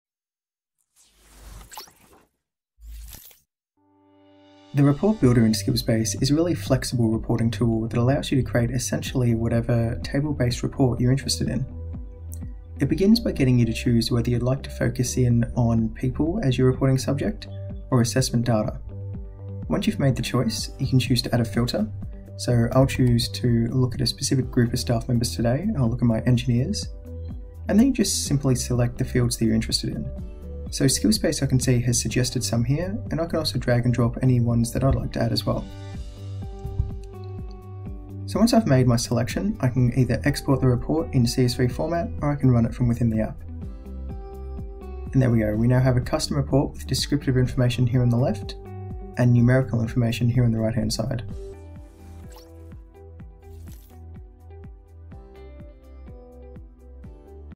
Skills Base - Report Builder
A demo of the Report Builder feature in Skills Base